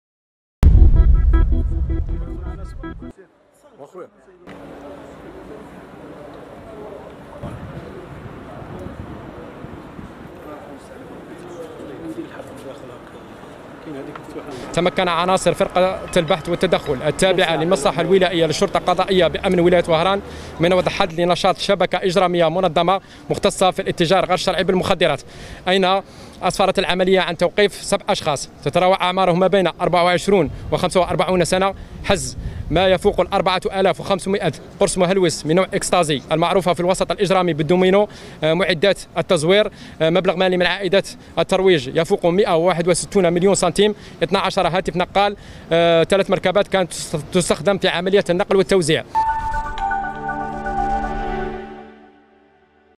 وهران: الإطاحة بشبكة إجرامية من سبعة أشخاص وحجز 4500 قرصا مهلوسا
وهران(وأج) - تمكنت مصالح الأمن من الإطاحة بشبكة إجرامية مكونة من 7 أشخاص، مع حجز 4500 قرصا مهلوسا.